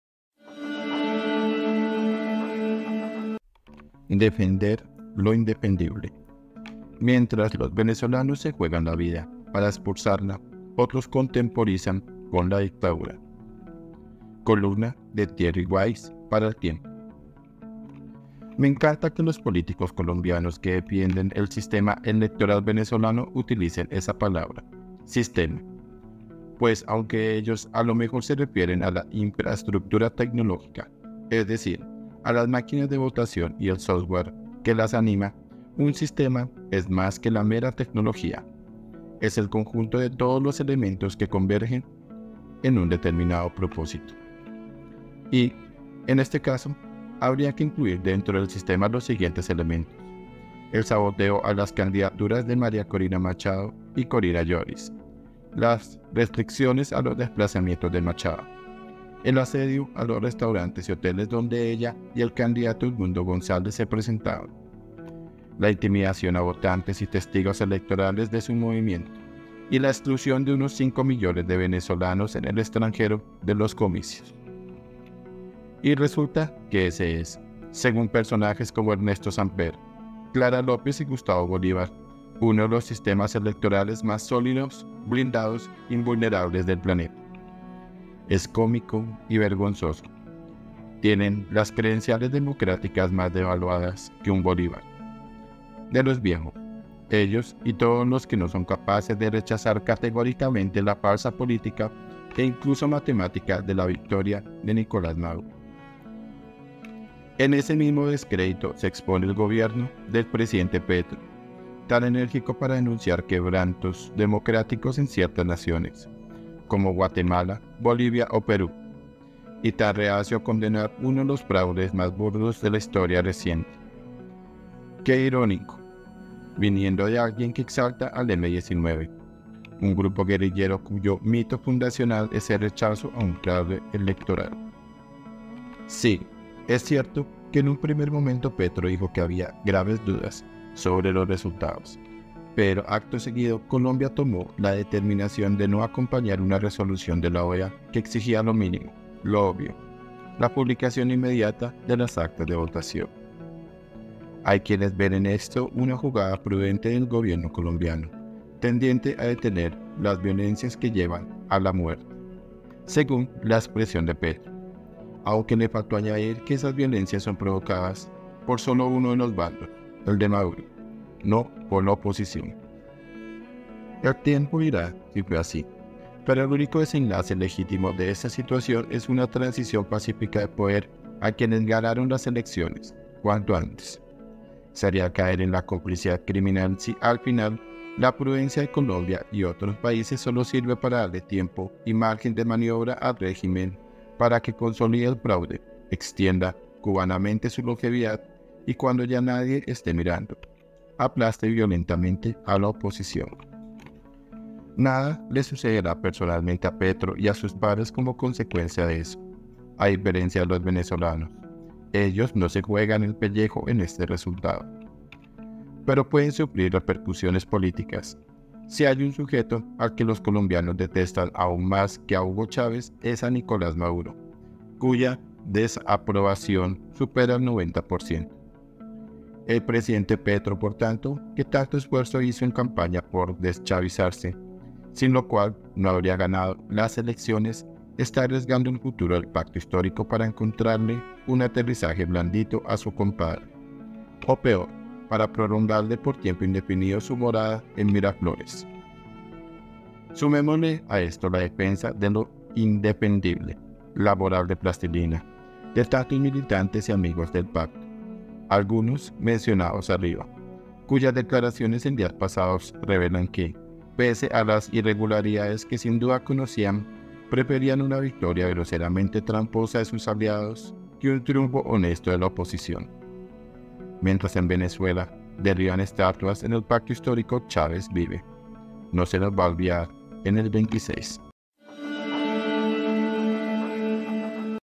DEFENDER LO INDEFENDIBLE 📢 Opinión de Thierry Ways
Me encanta que los políticos colombianos que defienden el sistema electoral venezolano utilicen esa palabra: ‘sistema’. Pues, aunque ellos a lo mejor se refieren a la infraestructura tecnológica, es decir, a las máquinas de votación y el ‘software’ que las anima, un ‘sistema’ es más que la mera tecnología: es el conjunto de todos los elementos que convergen en un determinado propósito.
columna THERRY WAYS para EL TIEMPO